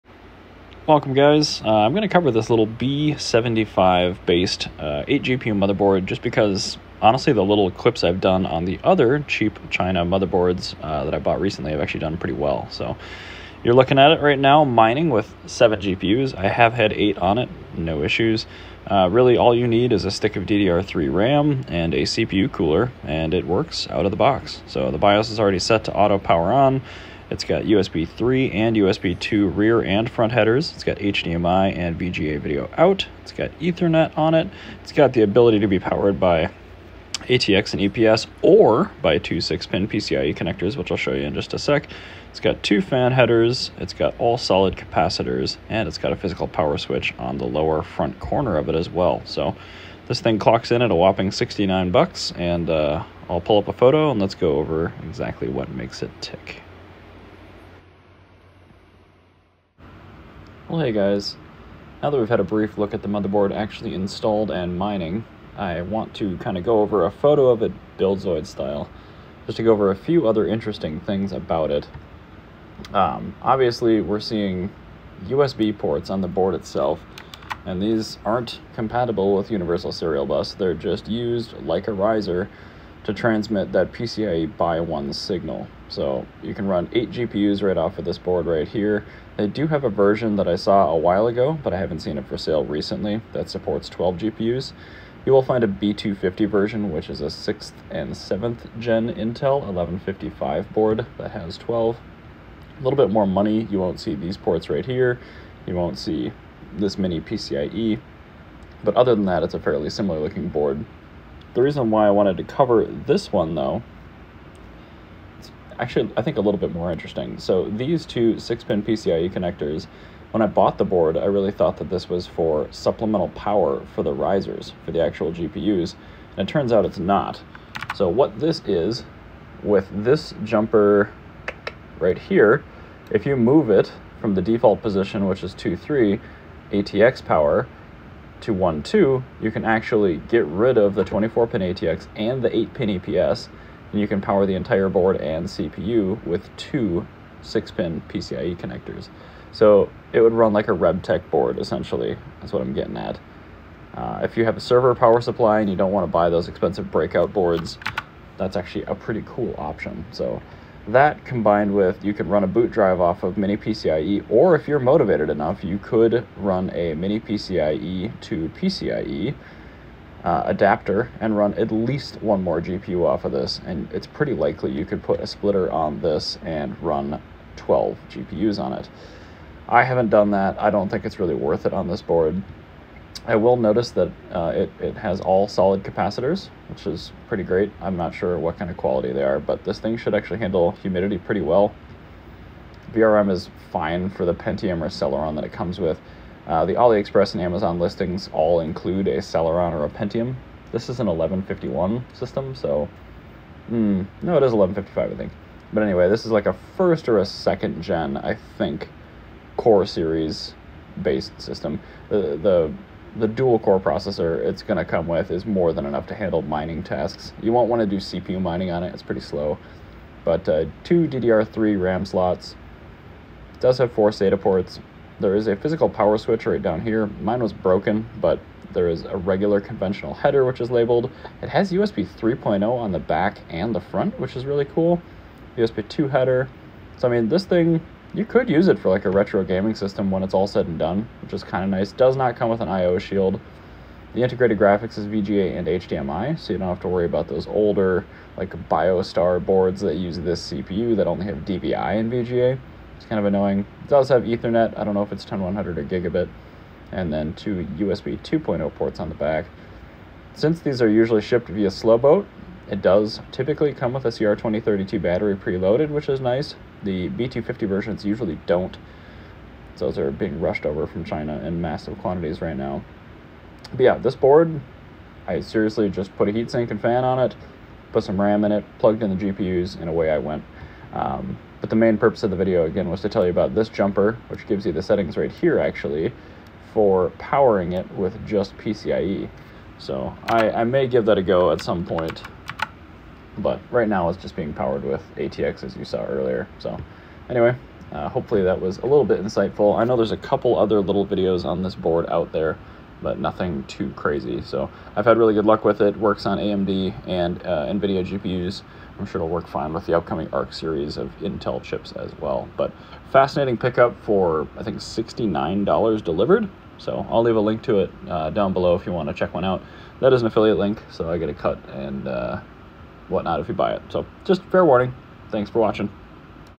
chinese b75 8 GPU LGA 1155 mining motherboard feature review
Motherboard with basic cooler
8GB DDR3 DIMM

this B75 based intel core series mining motherboard offers great value and efficiency for GPU miners small and large. 8 direct connect USB ports for GPU risers and a trouble free bios set to auto power on, PCIe 4G decoding enabled and Gen 2 speeds.

the motherboard also supports direct server PSU power, eliminating the need for a conventional power supply ATX unit.